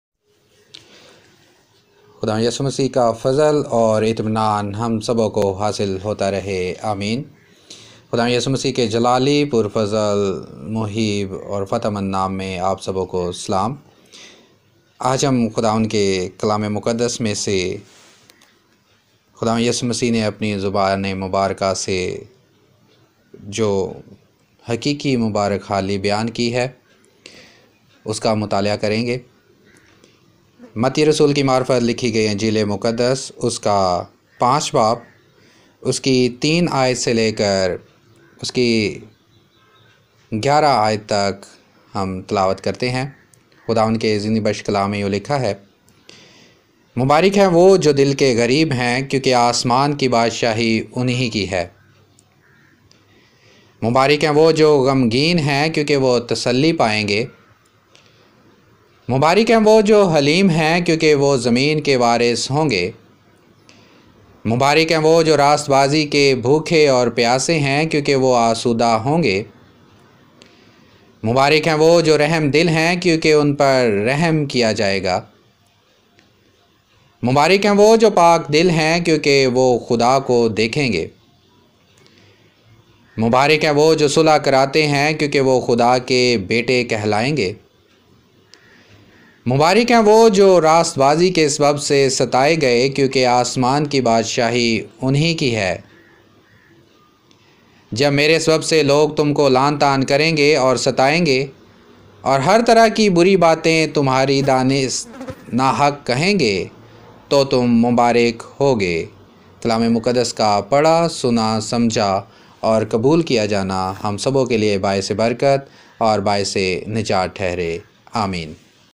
Teachings of Jesus Christ!! Methew 5: 3-11 !! 30 April,2021!! حقیقی مبارک حالی
Speaker: Baber Shahzad Gill
Chaman Colony Shahpur Sadar Sargodha